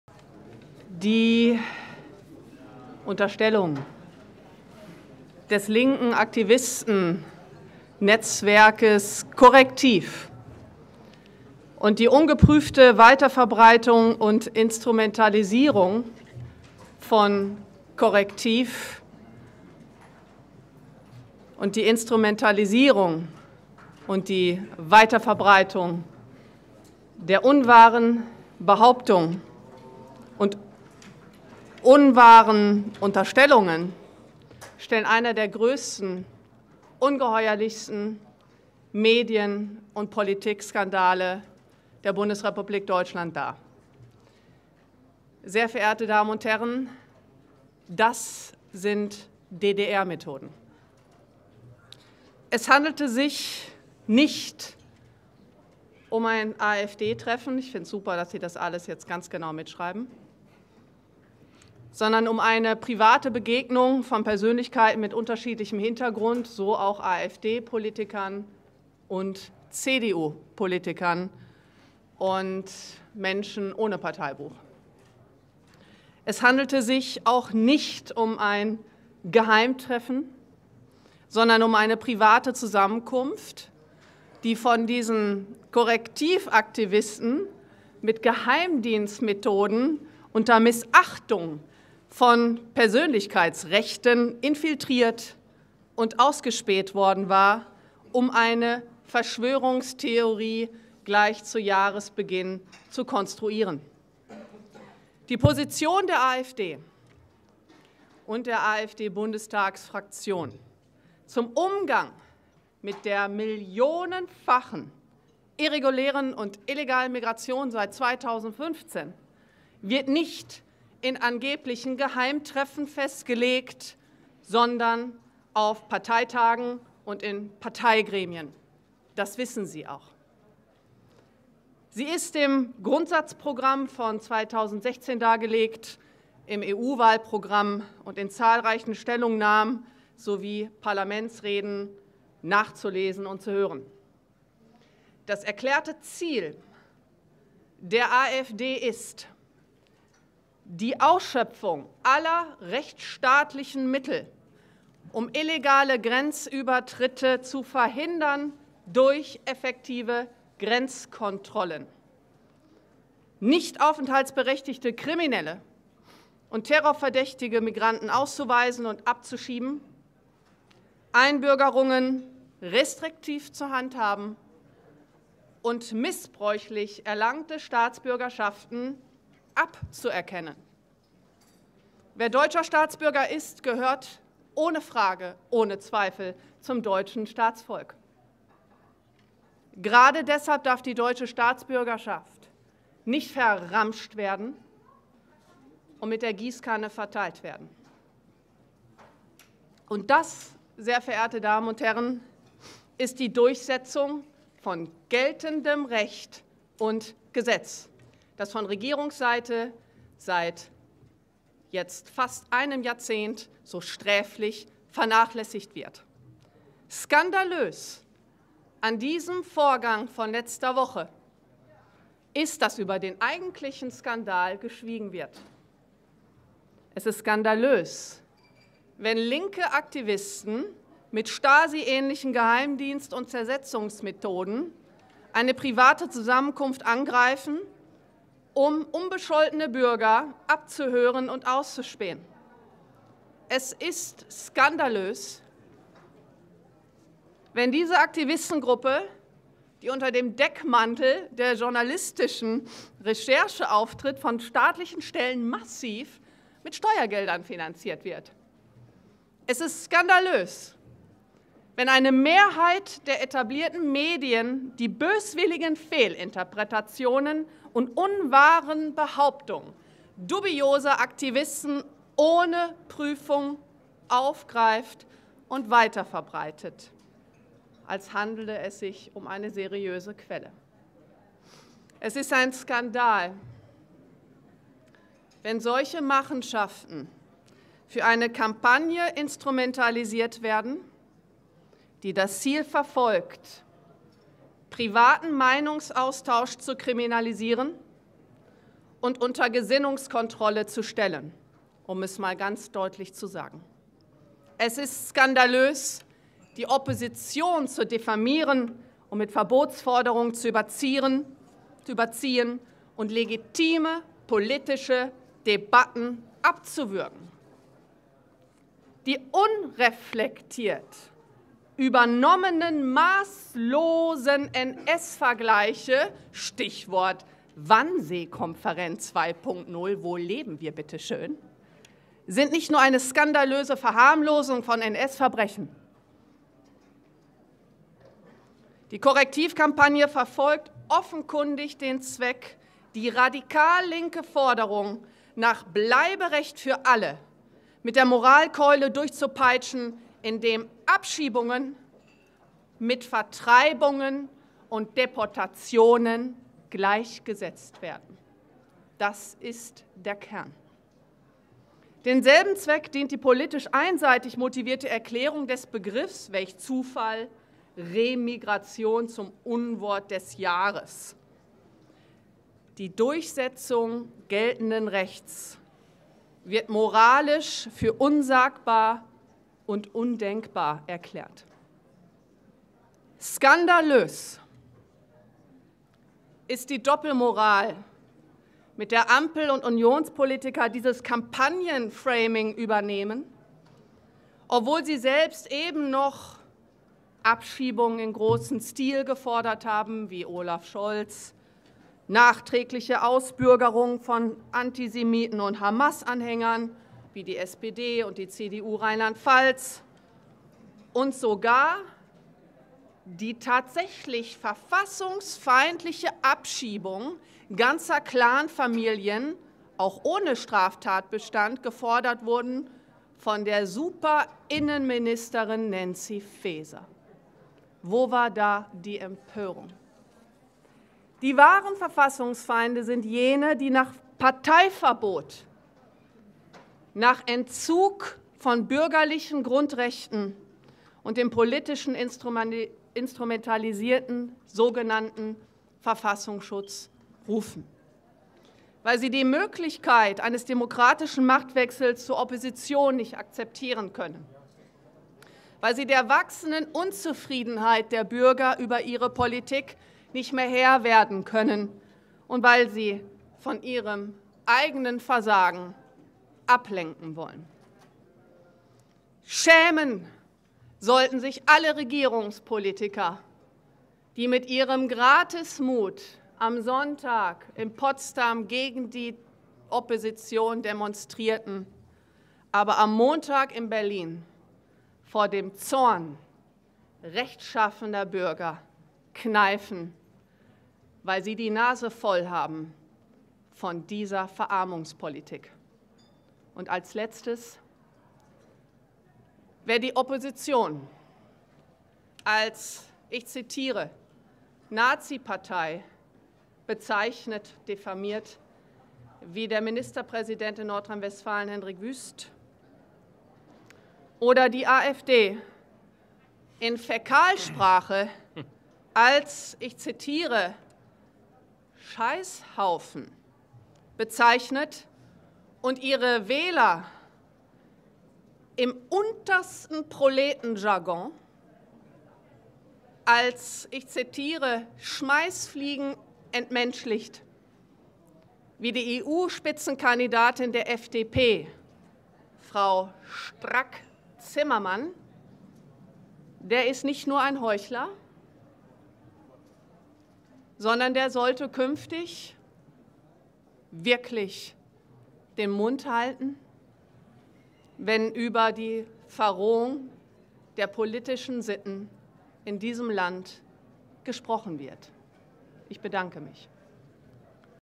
Correctiv & Medien vs. AfD: Die Wahrheit über die Schmutzkampagne!
AfD-Bundessprecherin nimmt Stellung zur inszenierten Schmutzkampagne durch das staatlich finanzierte Correctiv in Verbindung mit den etablierten Medien.